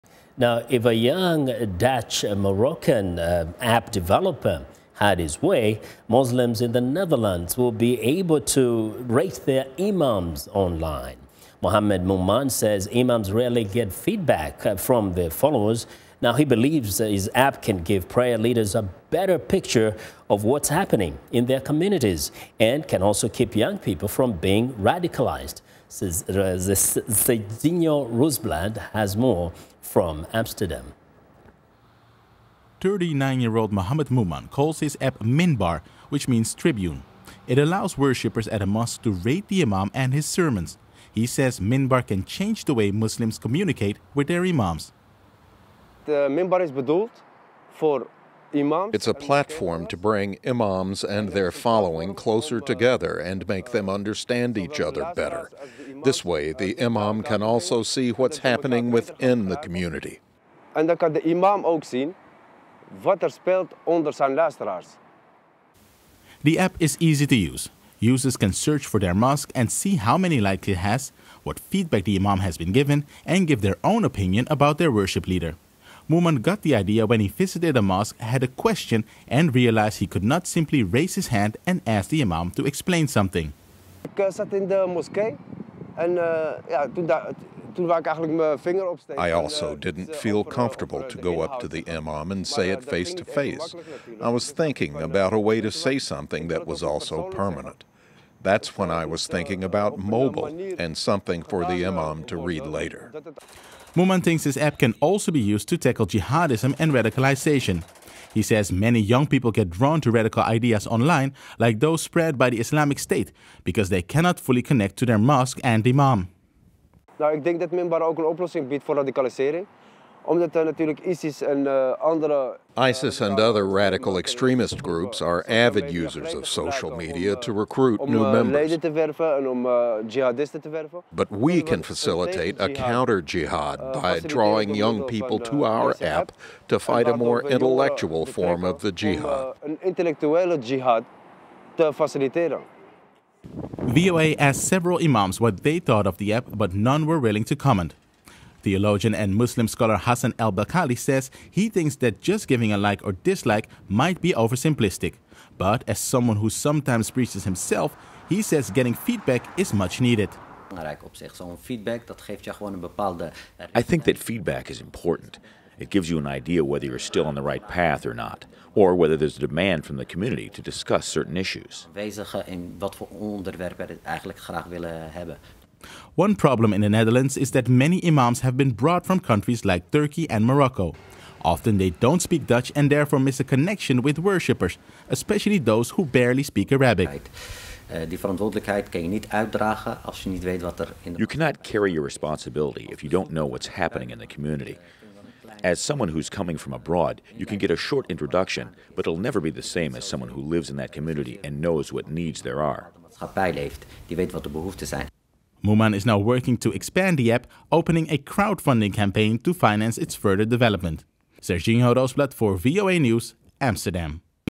Rate Your Imam App
If a young Dutch-Moroccan app developer has his way, Muslims in the Netherlands will soon be able to rate their imams on-line. Mohamed Mouman says imams rarely get feedback from their followers. Serginho Roosblad reports.